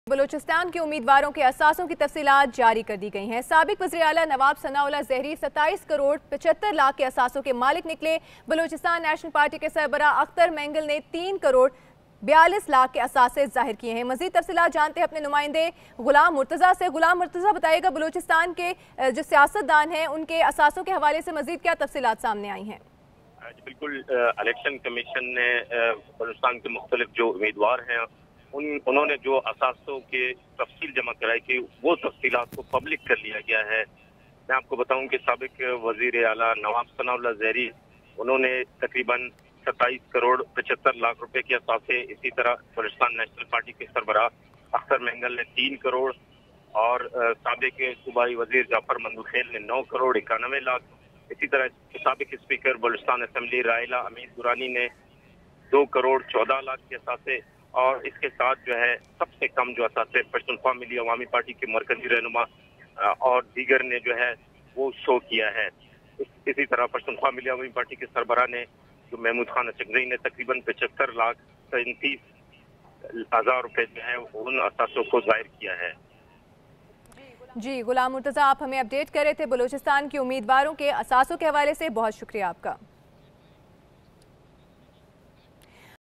Election commission issues assets detail of Balochistan Assembly candidates | Public News
Election commission issues assets detail of Balochistan Assembly candidates

Public News dutifully brings you news about the Public of Pakistan.
It's all about Zaban-E-Khalq.